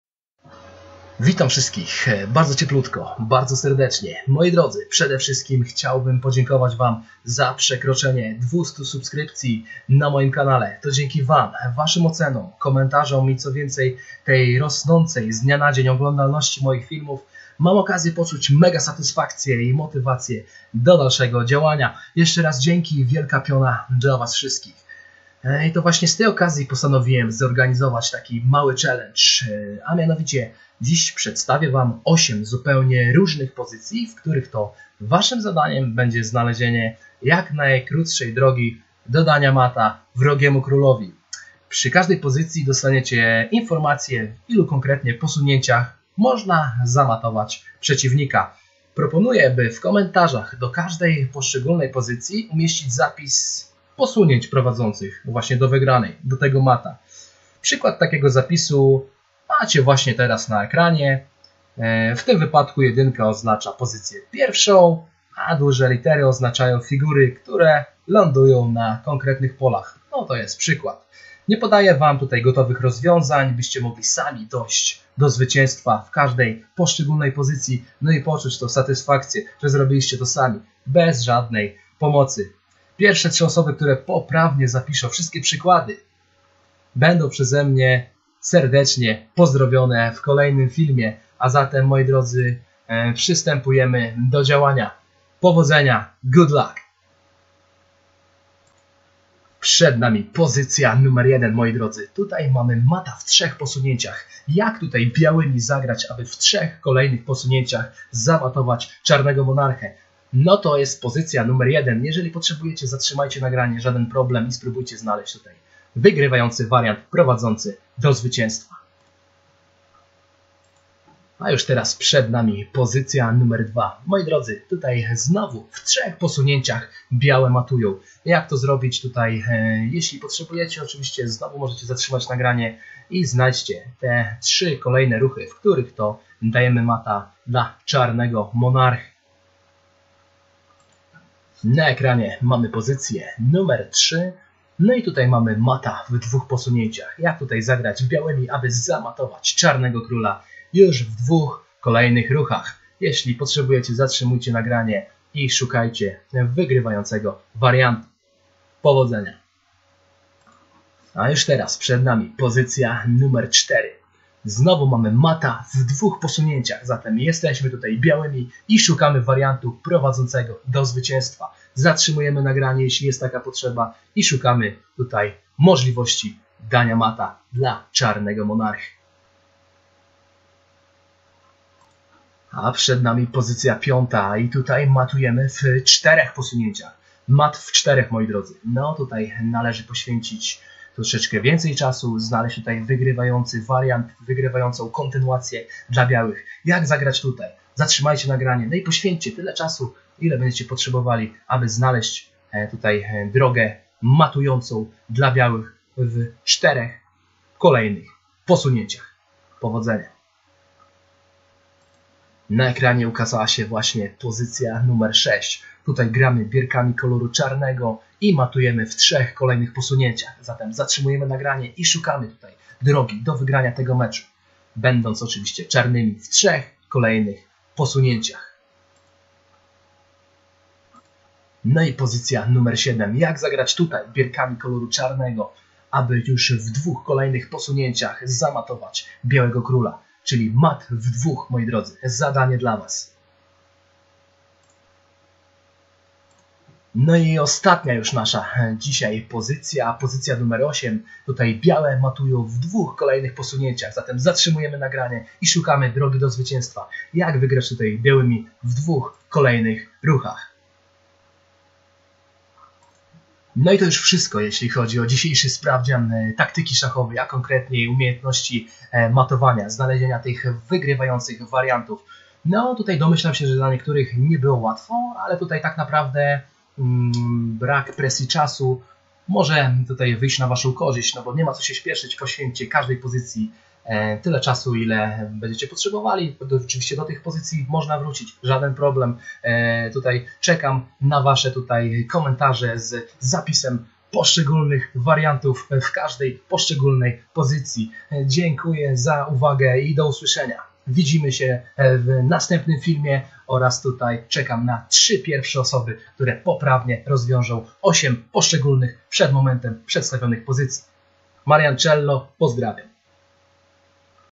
Sprawdź swoje umiejętności taktyczne i zamatuj wrogiego króla
Przedstawiam Wam kolejny filmik w którym to macie szanse sprawdzić swoje umiejętności taktyczne i spróbować znaleźć wygrywające kontynuację w 8 różnych pozycjach ! Przypominam o możliwości zapisania wygrywających posunięć w komentarzach ! Miłej zabawy ! :)